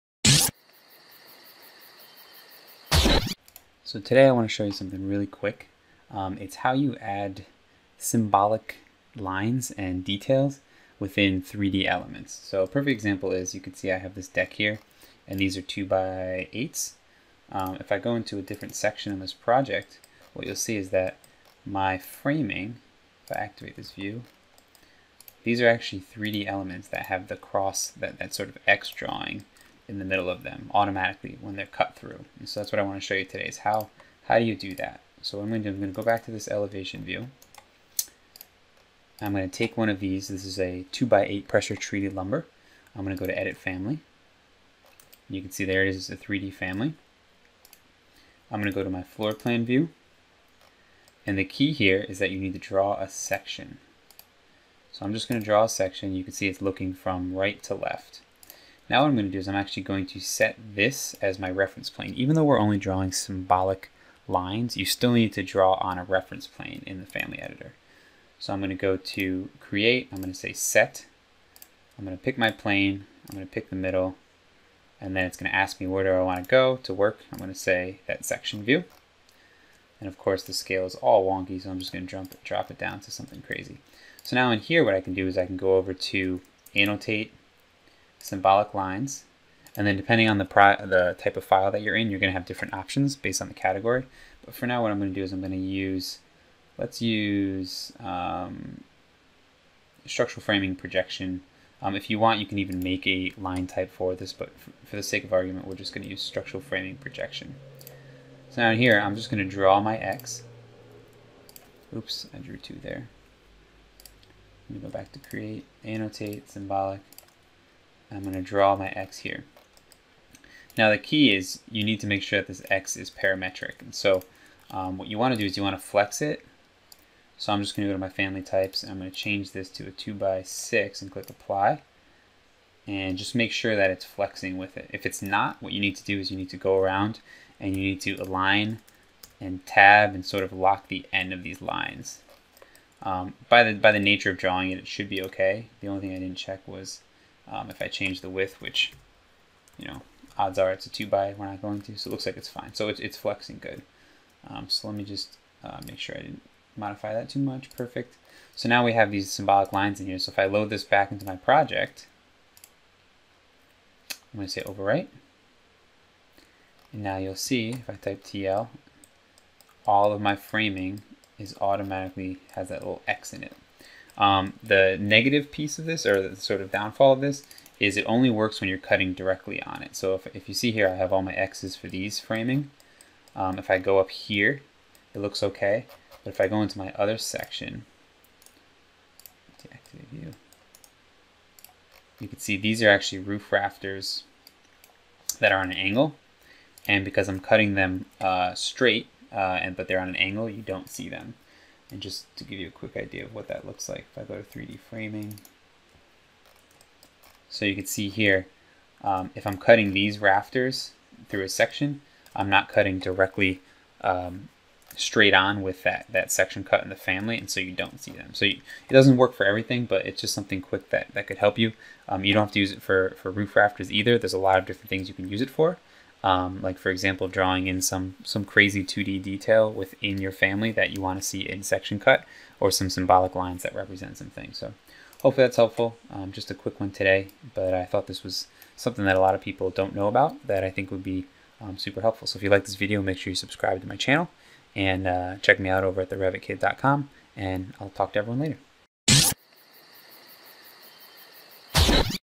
Add an "X" Symbol When Your Framing is Cut in Revit Tutorial
Really quick but powerful Revit tutorial about how to add an "X" symbol when using wood framing in Revit.  This can apply to any 3D family that is being cut and you want to show some sort of symbolic detailing... Imagine never having to place a wood stud "Detail Component" again??

My “Ultra Mobile” Revit Laptop (Gigabyte Aero 14)